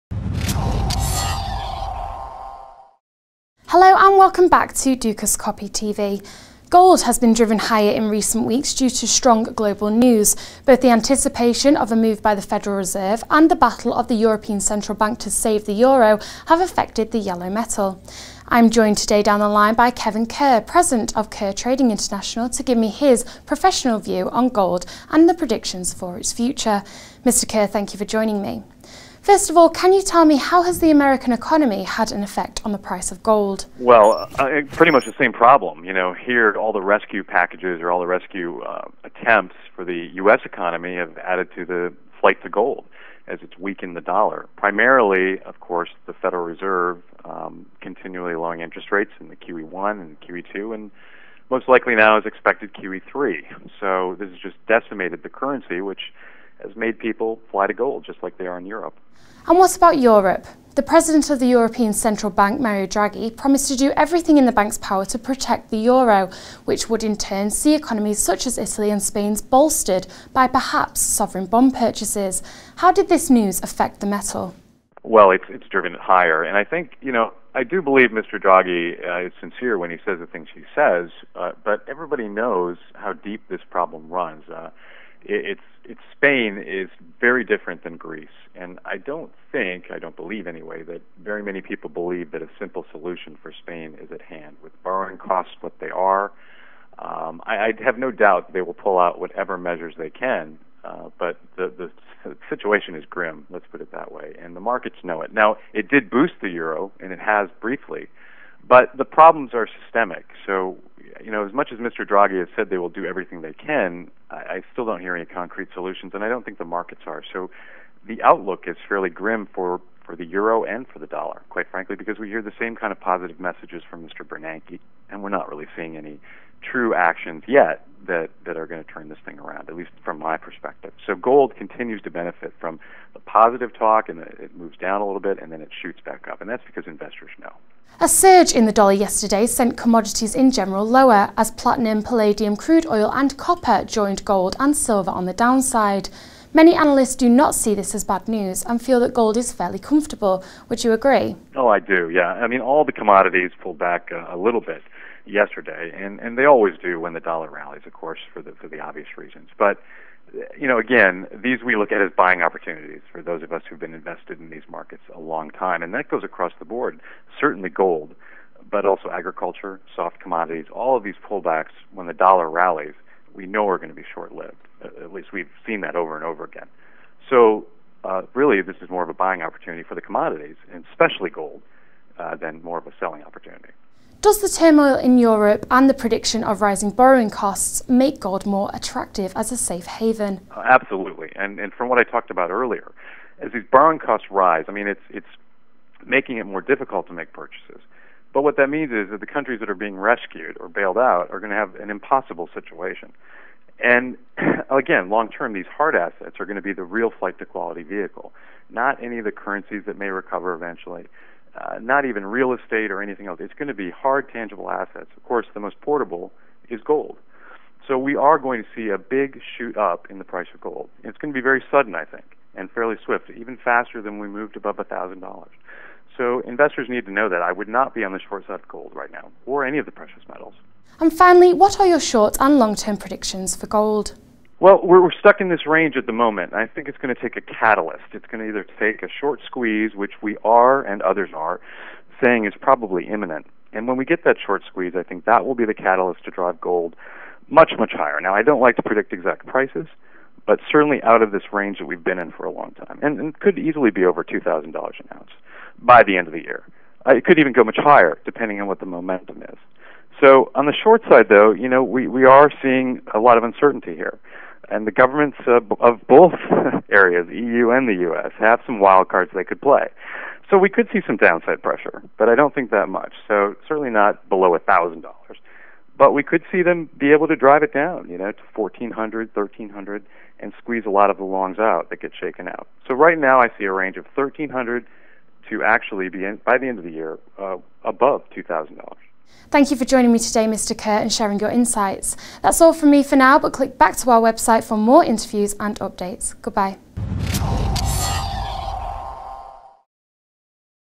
Kevin Kerr on Gold Fluctuations
Ahead of a week of data releases, Kevin Kerr CEO of Kerr Trading International talks to Dukascopy about the recent news from both the US and Europe causing Gold to shoot higher.